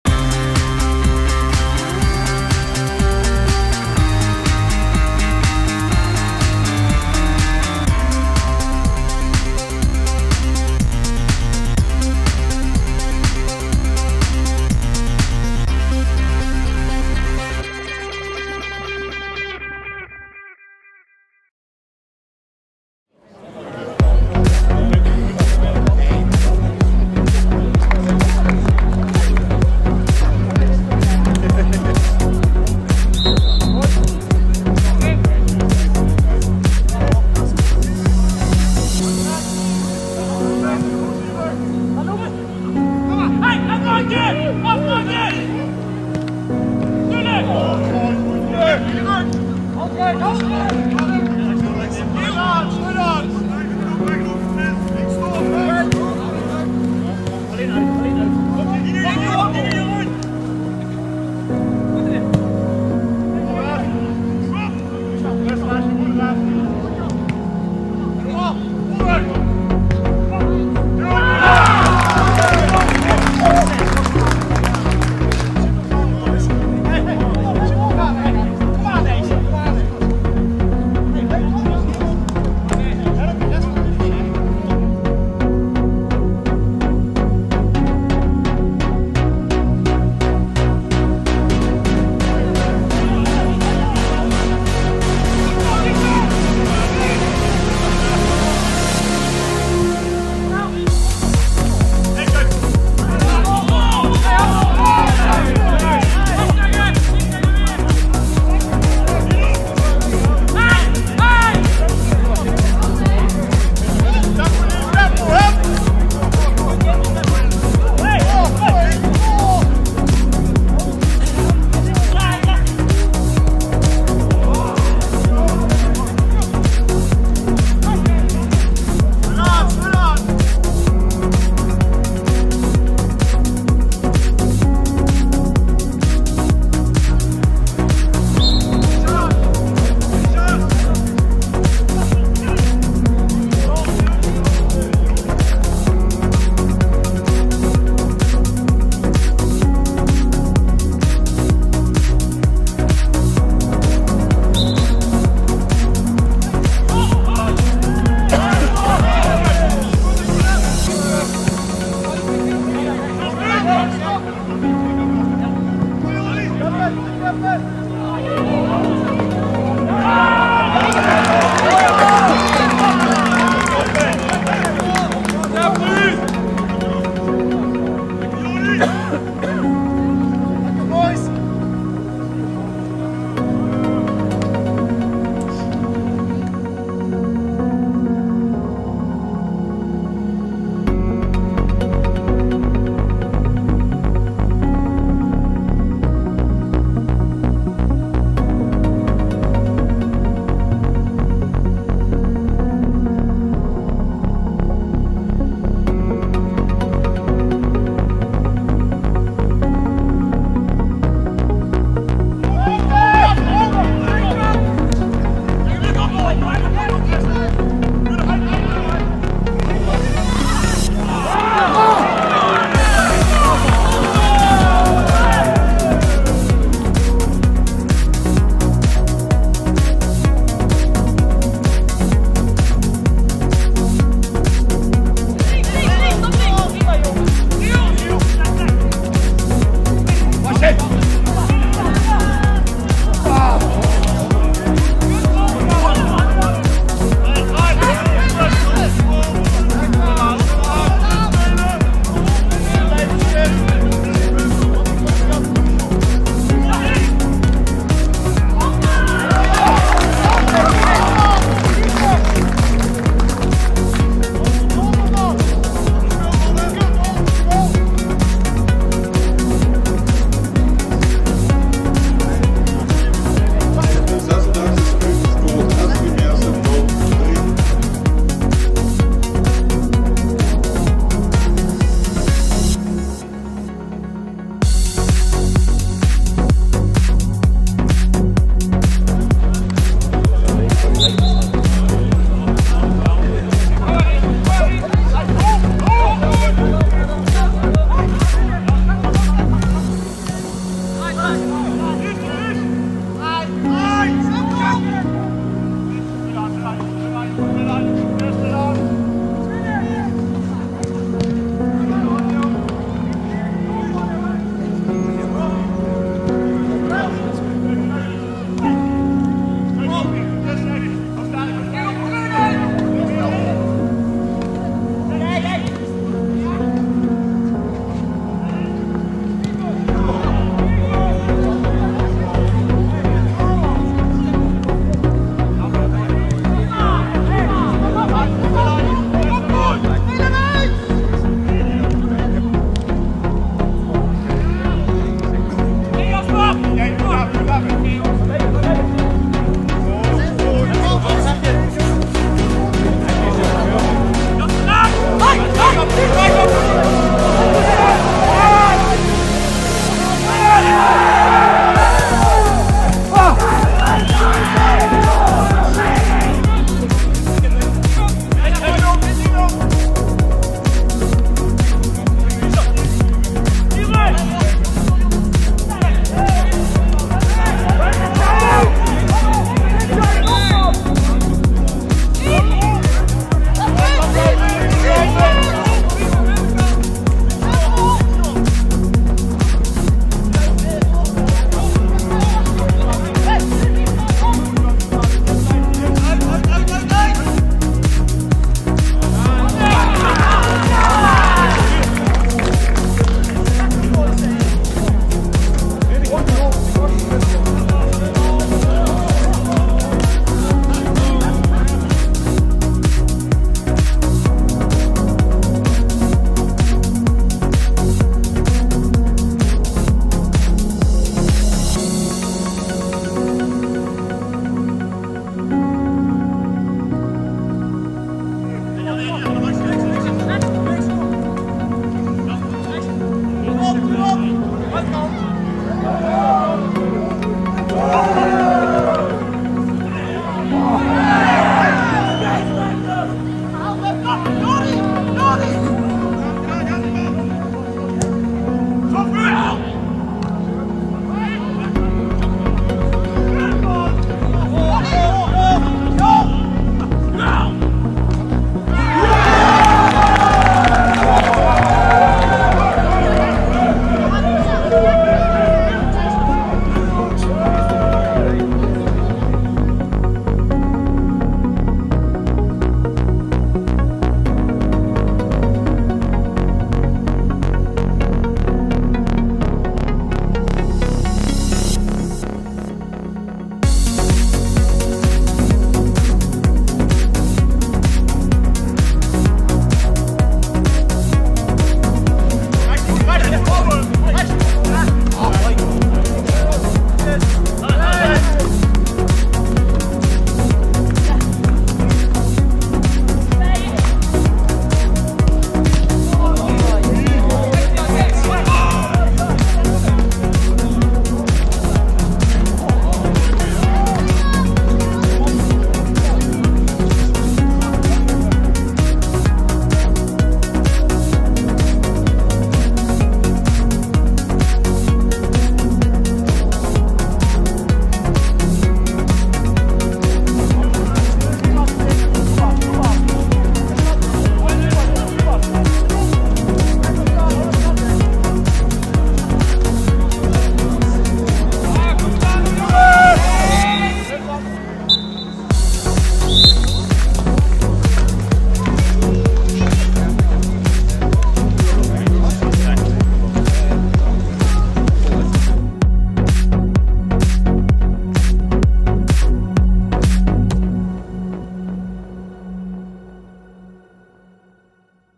Samenvatting bekerwedstrijd Sportclub'25-SV Meerssen 14-10-2021
Samenvatting van de bekerwedstrijd tussen Sportclub '25 (2e Klasse) uit Bocholtz en SV Meerssen (Hooffdklasse) van donderdag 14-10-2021